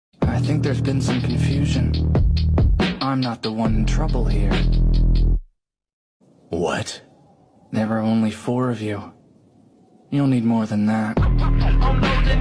I'm not the one in trouble here
4 bandits think they can take on the tavernkeeper